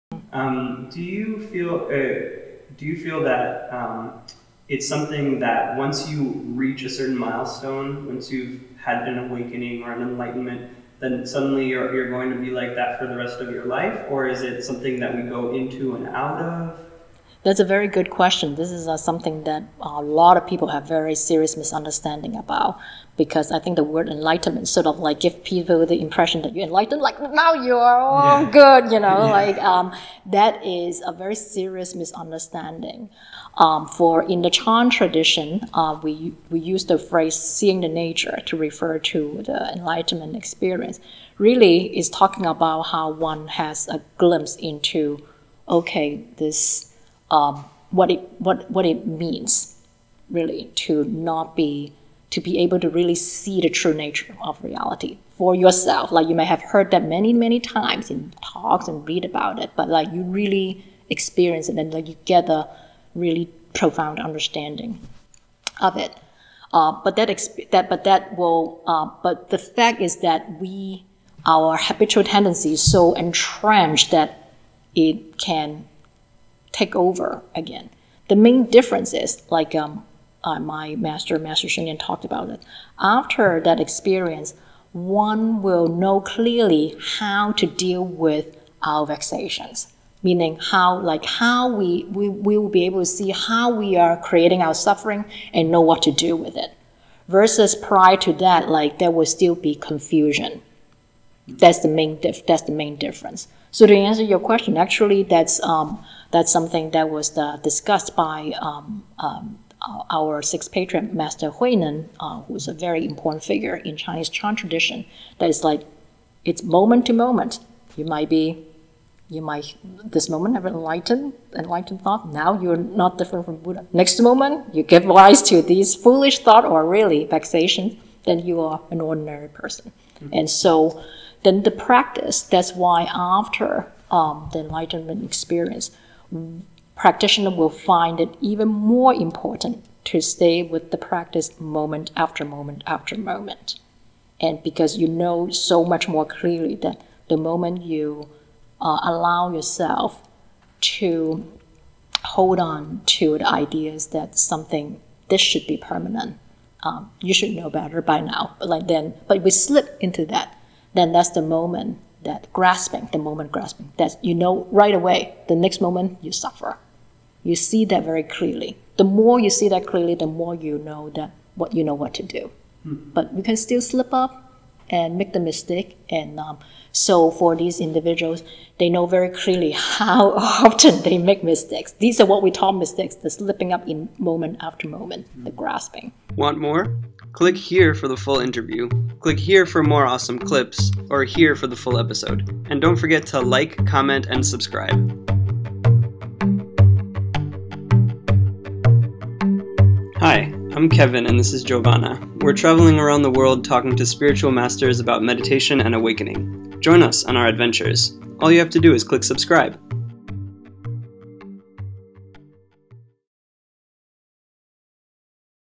Rebecca Li - Gaining and Losing Awakening
This is Rebecca Li, one of the Dharma Teachers at Dharma Drum Retreat Center in upstate New York, talking about what Happiness means to her. Check out Dharma Drum at
If you like this video, please donate to Dharma Drum to help Rebecca and her fellow teachers reach more students!